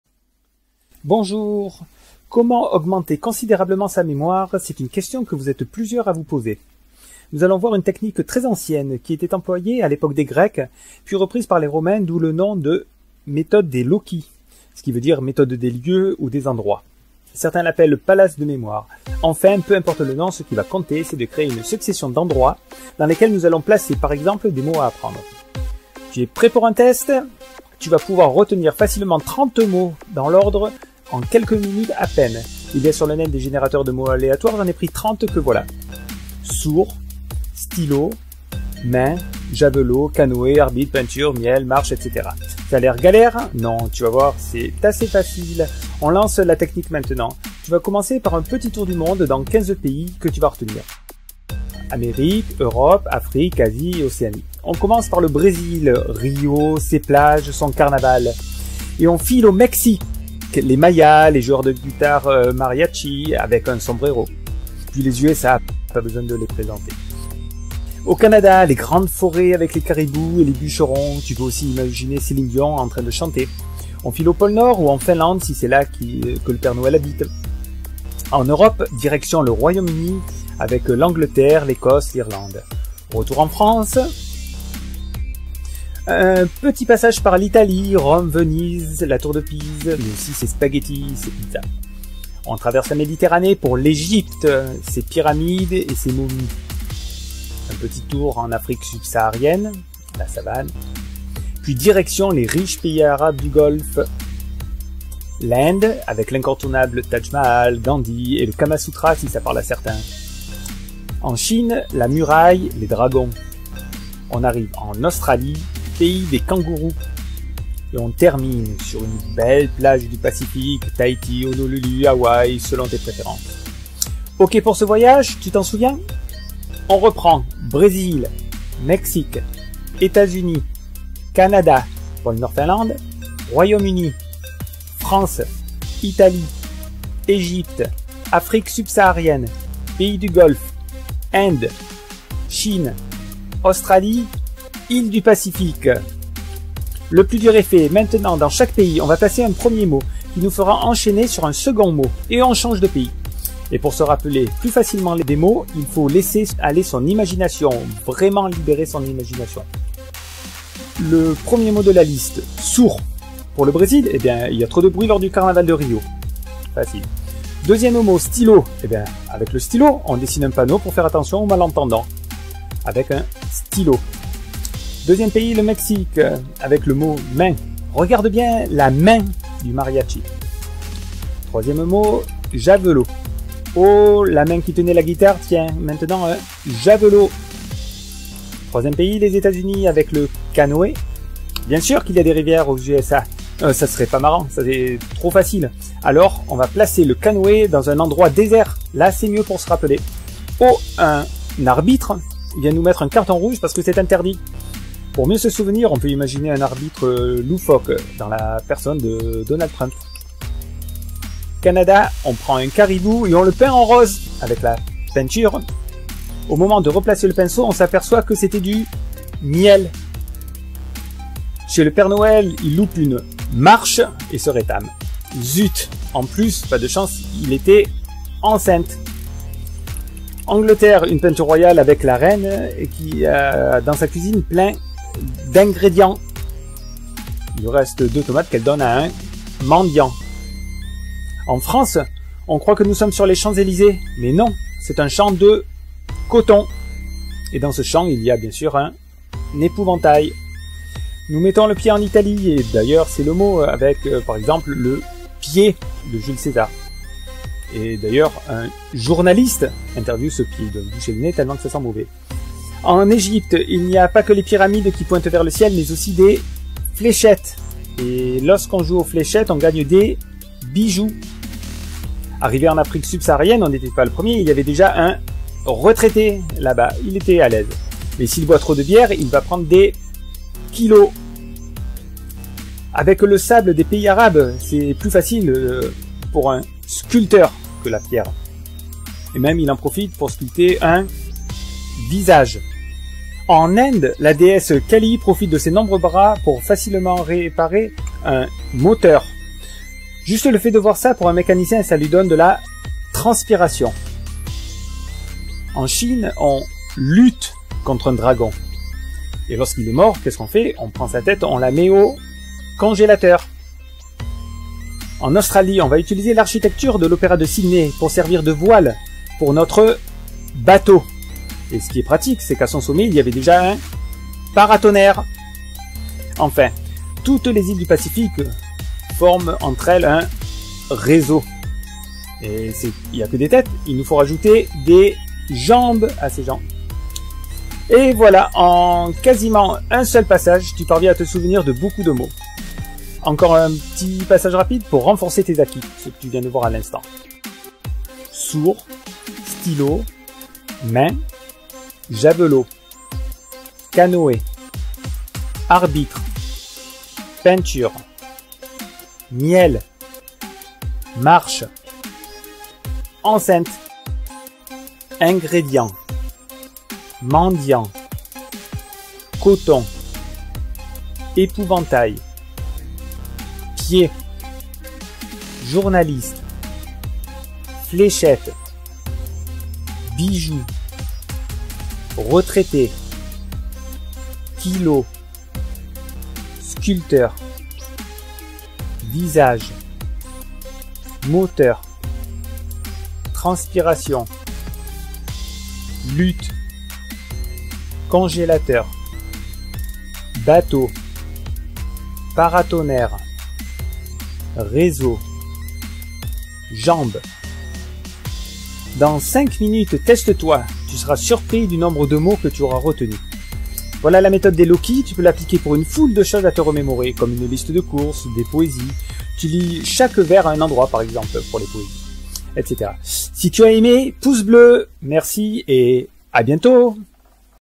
Méthode des loci (mémorisation) [1/2]
En quelques minutes, apprenez à retenir des dizaines de mots, très facilement grâce à la « méthode des loci » (lieux) ou « palais de mémoire » : construisez votre cheminement et placez-y les mots à vous rappeler.